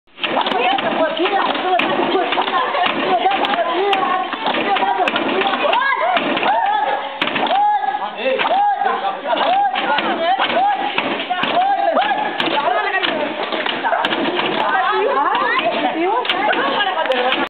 Ferme Ecole de Fianaratsoa
FR/
Battage de riz à la Ferme Ecole de Fianaratsoa.

ES/
Trillado de arroz en la Granja Escuela de Fianaratsoa.